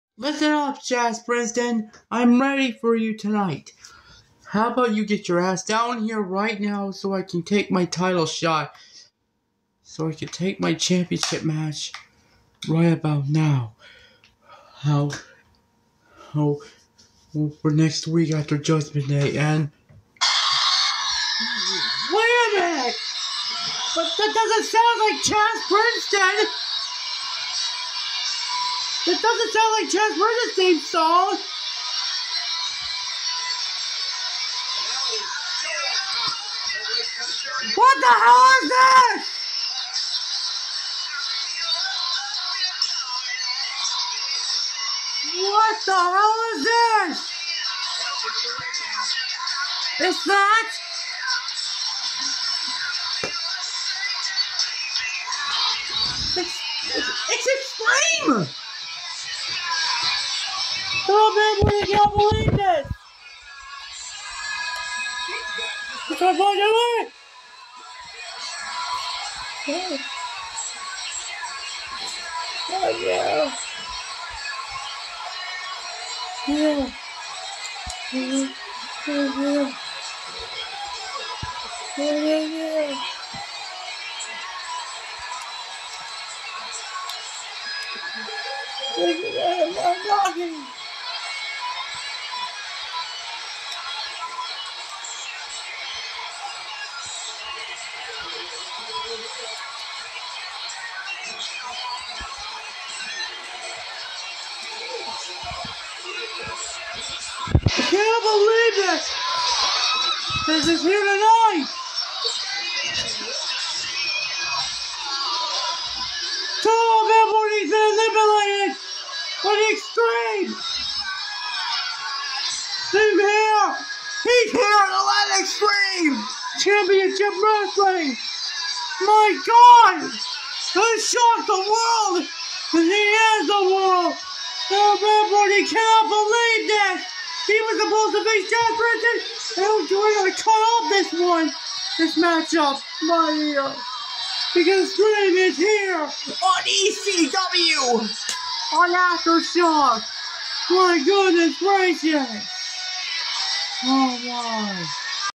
ECW Aftershock Episode 21 Part 5
Originally was supposed to be Chazz Princeton vs Totodile Fan 14 But Someone Has Arrived